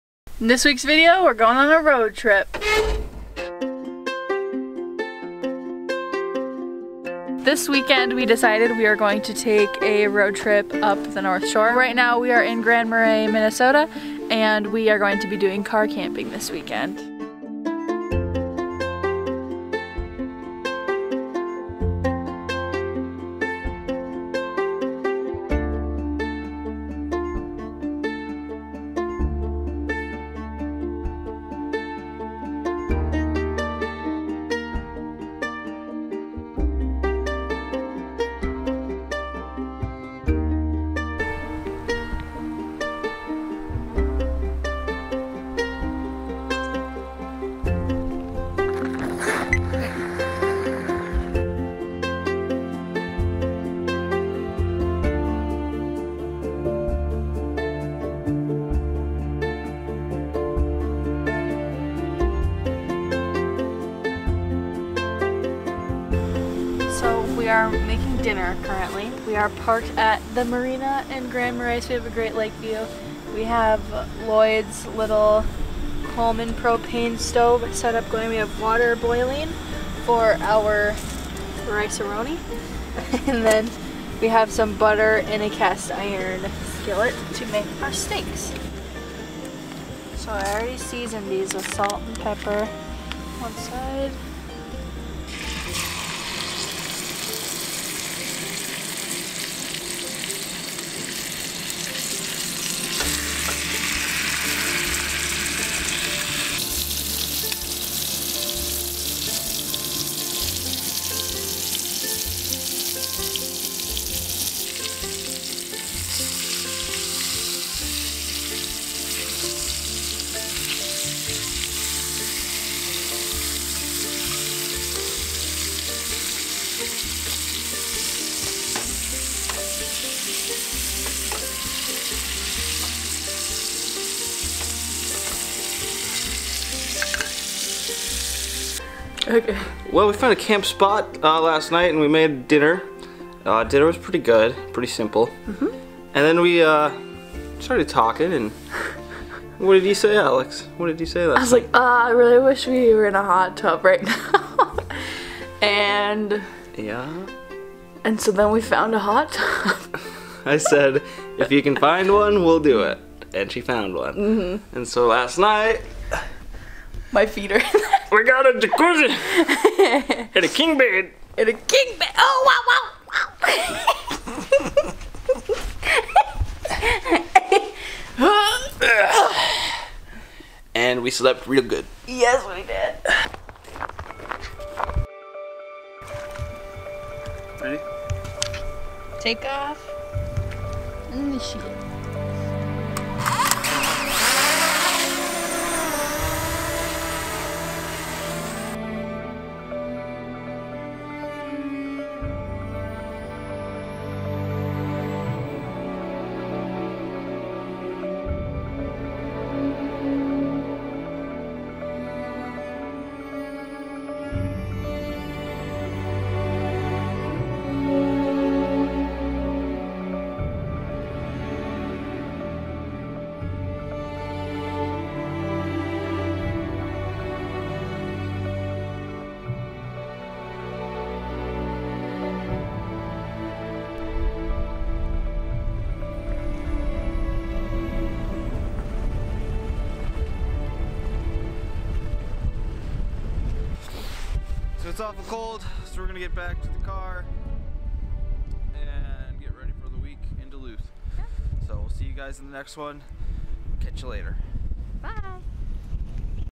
Winter Weekend on the North Shore of Lake Superior
This week we venture north to experience the tranquil nature of Lake Superior. This video features drone footage of the shore, wildlife from the long lens, and a brief vlog of our adventure. We certainly enjoyed getting some peace and quiet out of the big cities. Another van-build video to come next week!